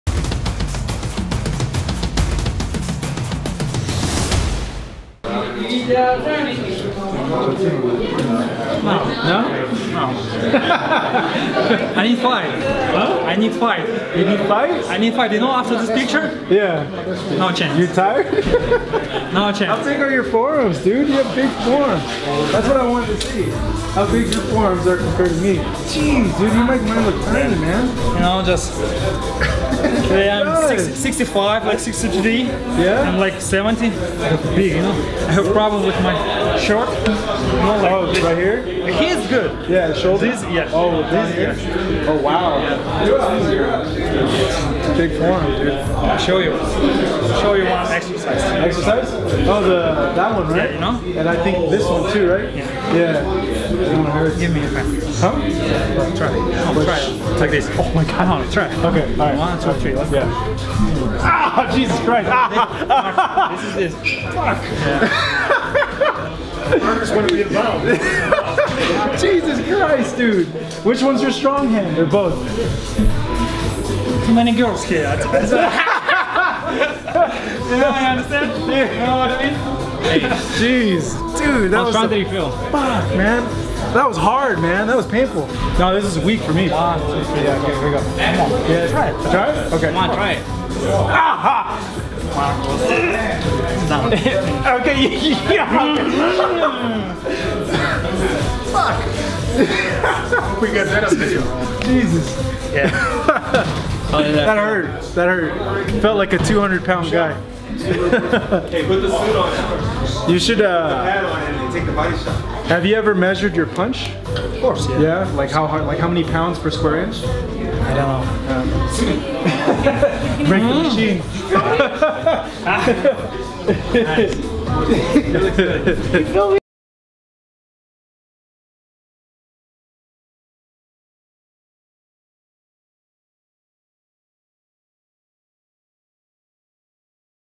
Gennady Golovkin almost BREAKS REPORTERS HAND! Displays UNREAL grip strength!
Gennady Golovkin almost breaks a reporters hand! As GGG asks him to test out his grip strength and nearly crushes his hand with brute strength! As Golovkin shows how strong his grip is!